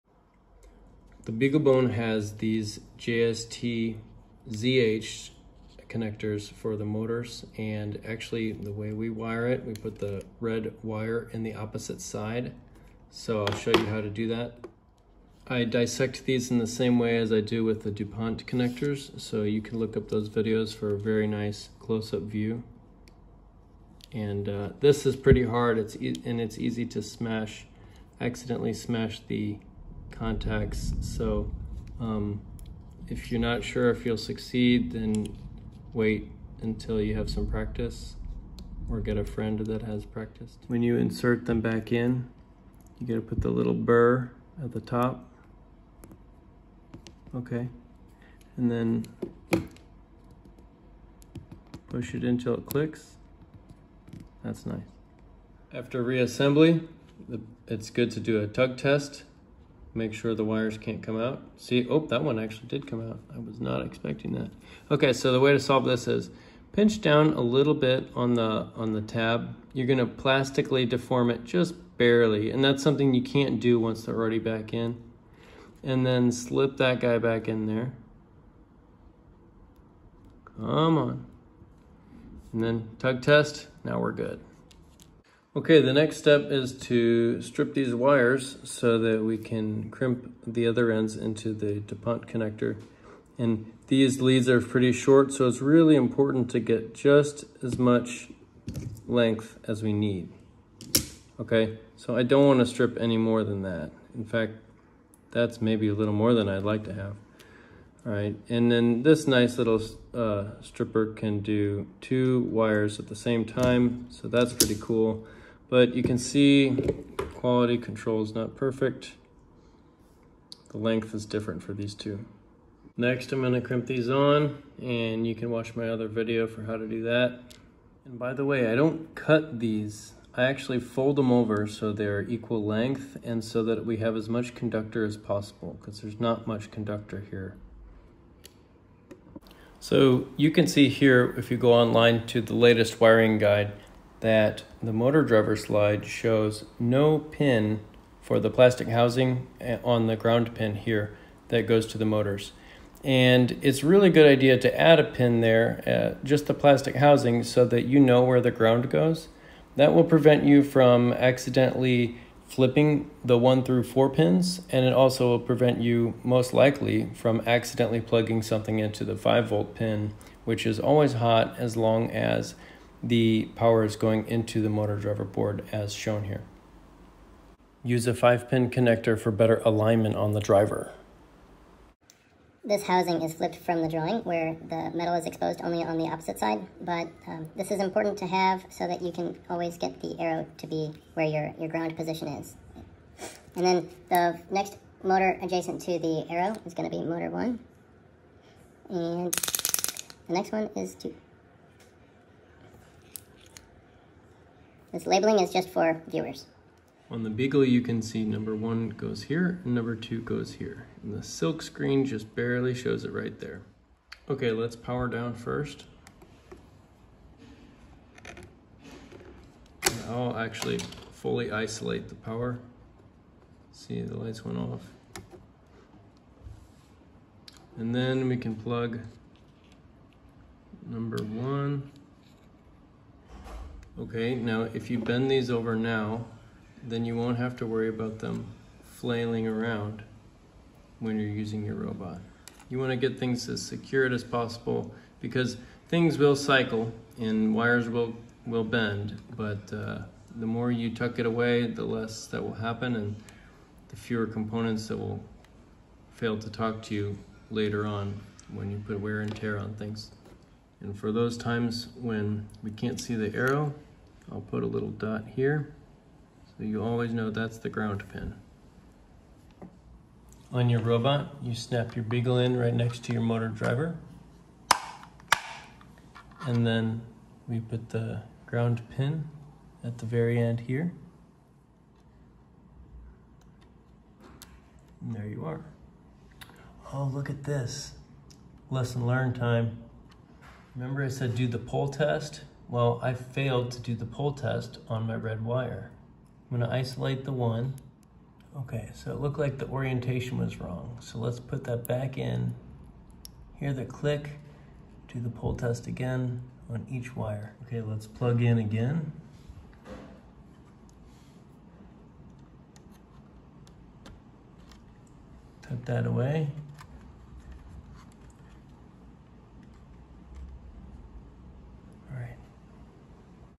SCUTTLE Robot - Build Motor Driver Signal Wires JST-ZH Macro View for Beagle
This video shows in painstaking detail how to build your motor driver signal wire harness which consists of 2-pin connectors attached to the beaglebone and a 4pin dupont connector which connects to the driver board.  These wires carry PWM signals to the h-bridge.